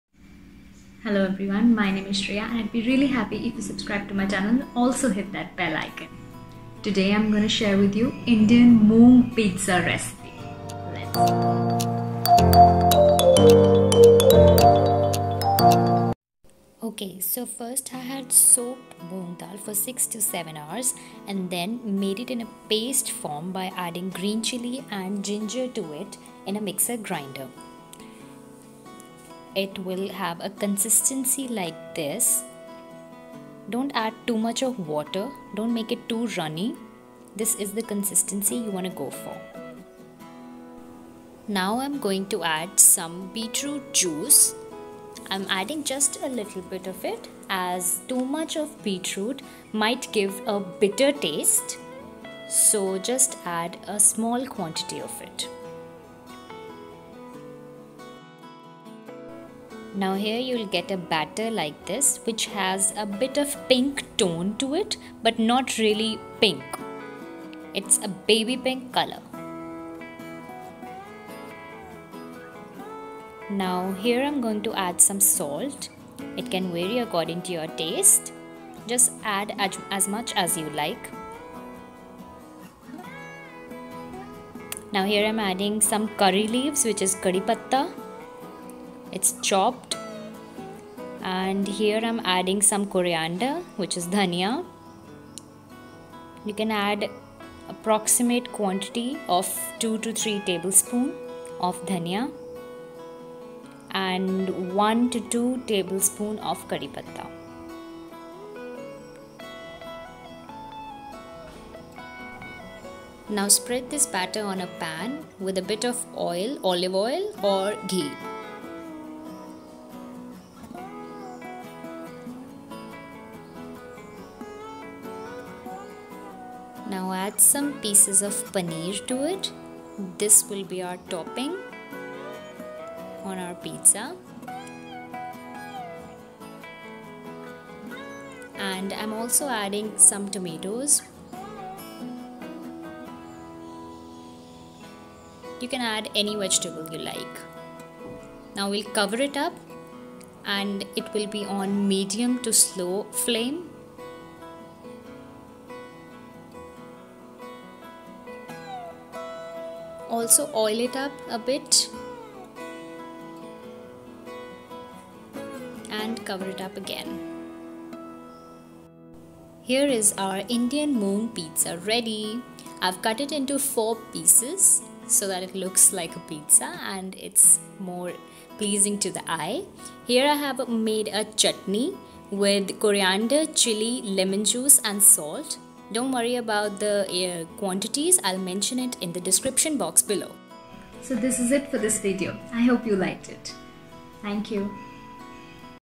Indian moong Pizza | Beetroot moong Pizza
Hey lovelies
Today I've got you a recipe which is filling, tasty and as well as nutritious. Make it in breakfast, lunch or at the snack time. It's the Indian moong pizza.  I'm sure it will not disappoint you. Just try it once.

Moong dal (soaked)- 1 cup
Ginger - 2inch piece
Green chillis- 2
Beetroot juice- 2-3 tbsp
Salt- according to taste
Curry leaves (kadhi patta) 8-10 leaves
Coriander (dhania) chopped  - 4-5 tbsp

For chutney:
Coriander (chopped) - 10-12tbsp
Green chilli's- 2

Enjoy your quarantine time!
Stay safe❤️